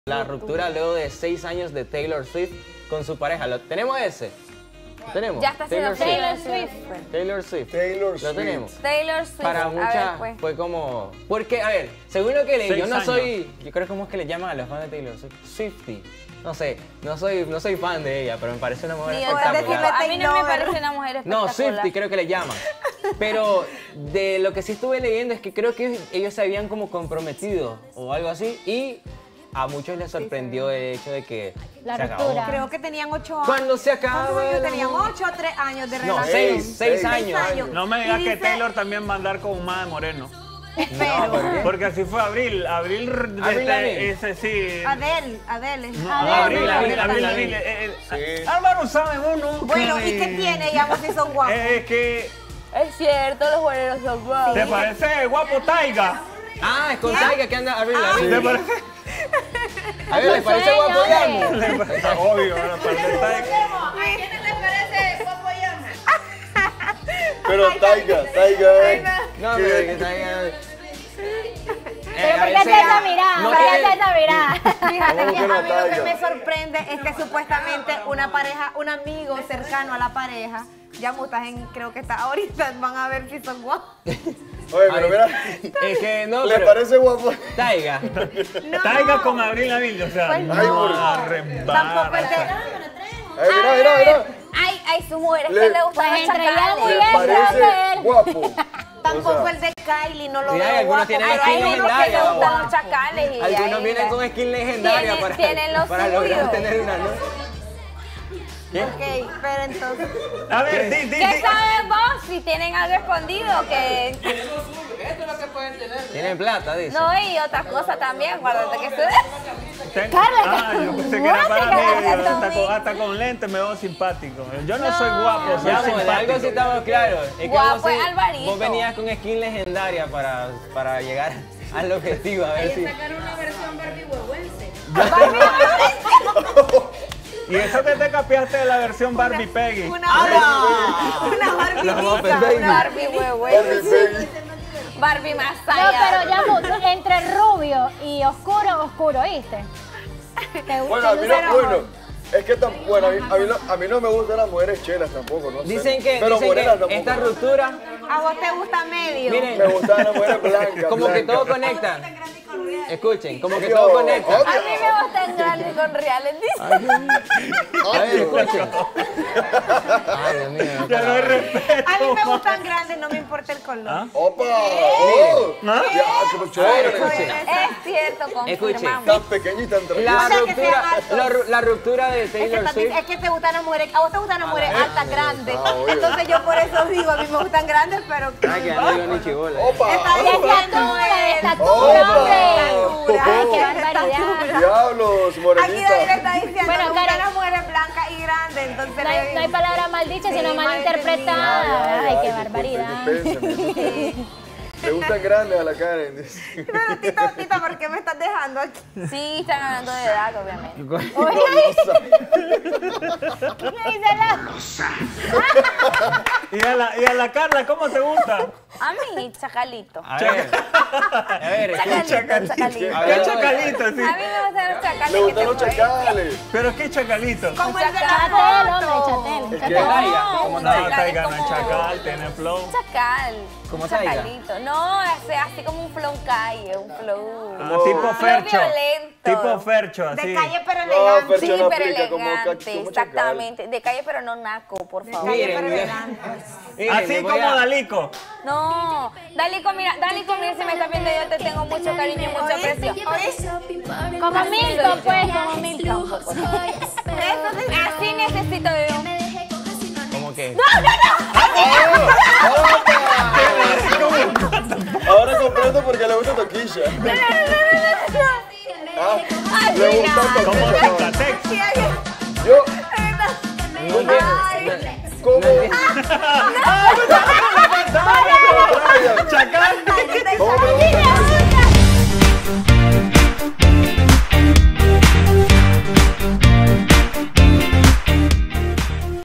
Taylor Swift y Joe Alwyn terminan su relación después de 6 años
Después de seis años de noviazgo, varias canciones dedicadas, diversos medios informaron la que la cantautora Taylor Swift y el actor Joe Alwyn terminaron su relación de manera amigable.